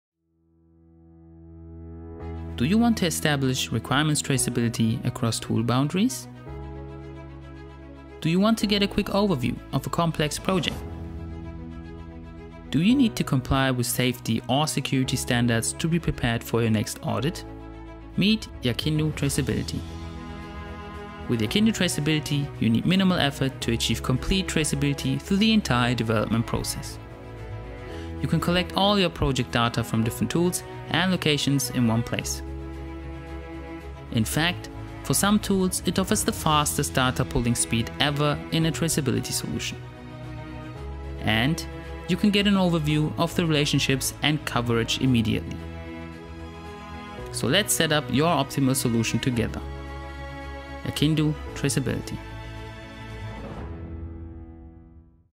Requirements Traceability made easy with YAKINDU
YAKINDU Traceability trailer video.

Be prepared for your next audit and improve your software’s quality by setting up the traceability for your project within hours.

YAKINDU Traceability is a professional requirements traceability management tool. It adapts to your usual development tools and creates a homogeneous traceability graph, without interfering with your established workflows.